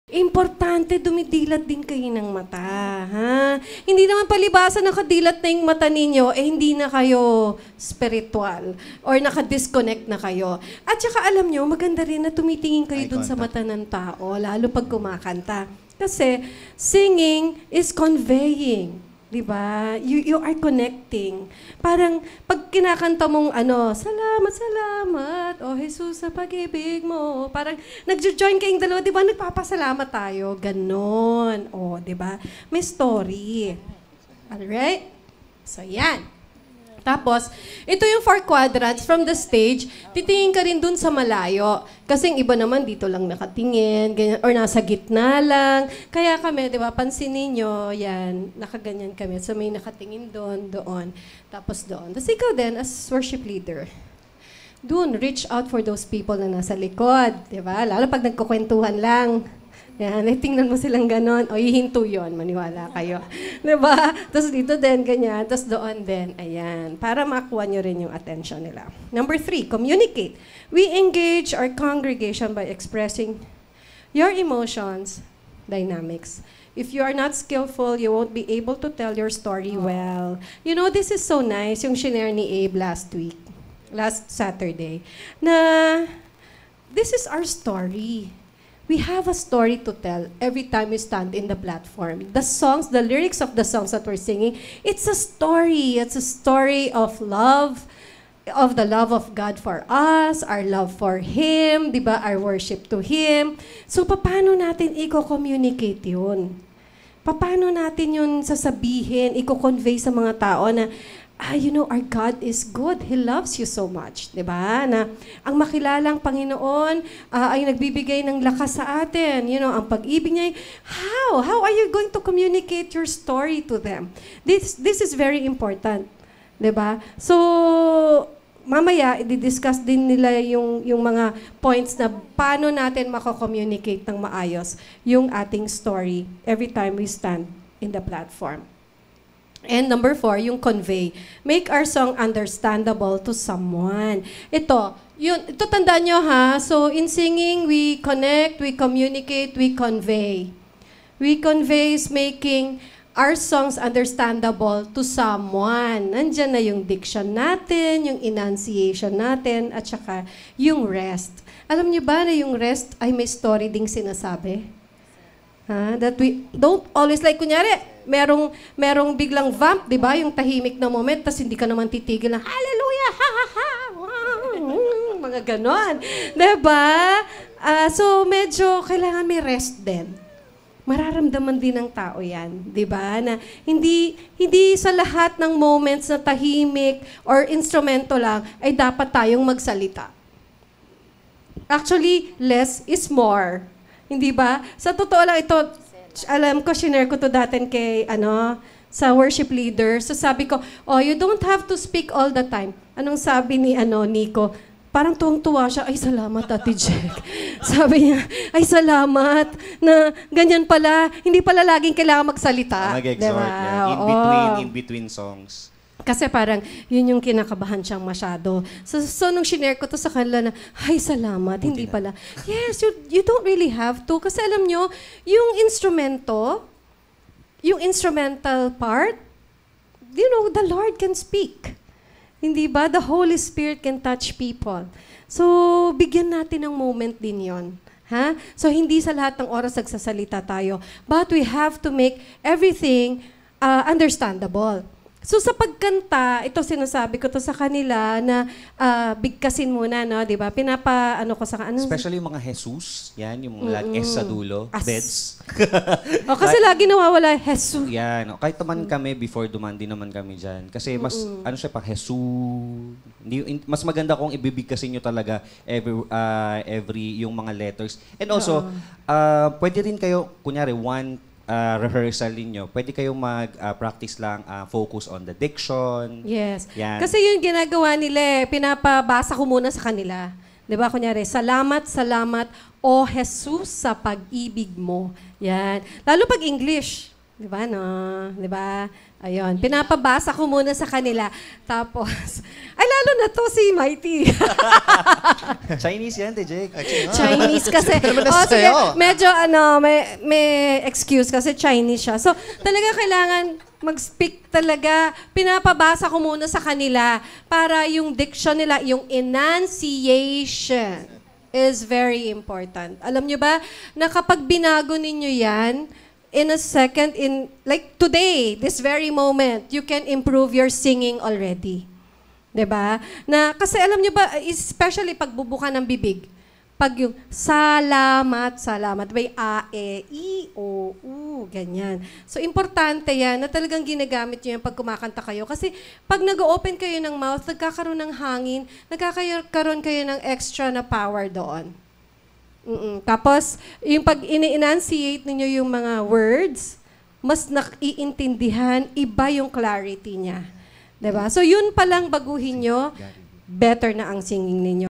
"HOW TO IMPROVE YOUR WORSHIP LEADING SKILL" by Jeck Cadeliña
This video which was taken during the MP WORSHIP LEADER MASTERCLASS will inspire and encourage you how to be a better worship leader. Grace wins!